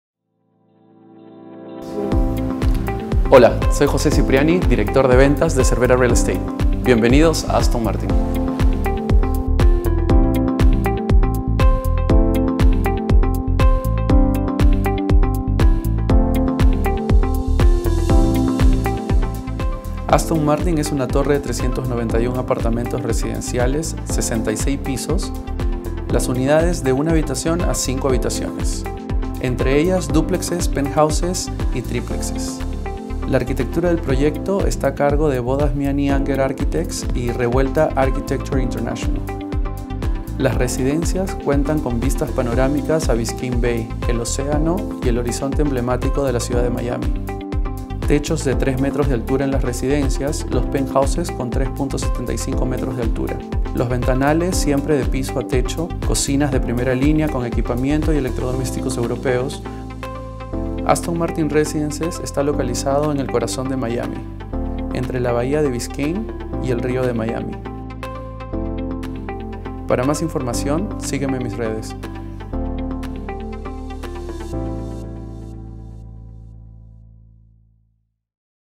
PRESENTACIÓN ASTON MARTIN RESIDENCES
JOSE CIPRIANI, PRESENTA PROYECTO ASTON MARTIN RESIDENCES - MIAMI - FLORIDA

Aston Martin Residences en 300 Biscayne Boulevard Way combina una magnífica estética de diseño con una ubicación ideal para crear la dirección más atractiva y codiciada de Miami.

Construida en una de las últimas parcelas disponibles de propiedad frente al mar, Aston Martin Residences en 300 Biscayne Boulevard Way es una sinfonía de vidrio curvilíneo y acero, irguiéndose en 66 pisos hacia el cielo.

De sus 391 residencias amplias y luminosas, la mitad cuenta con diseños de planta completamente únicos. Apenas un selecto grupo tendrá acceso a penthouses capaces de robarle el aliento a cualquiera – incluyendo la joya de la corona, un condominio triple verdaderamente impresionante.

Toda una variedad de amenidades, como un moderno gimnasio de doble altura y spa, amplia piscina, salones de belleza, lounges y una extraordinaria marina para súper yates se encuentran disponibles para su total comodidad. Con un diseño arquitectónico que le rinde homenaje a las líneas emblemáticas y elegantes de sus automóviles, Aston Martin vislumbró una estructura que capta perfectamente el espíritu emocionante y atractivo por el cual es mundialmente reconocido.

Oficina Lima - Perú
Ofic: 710-4243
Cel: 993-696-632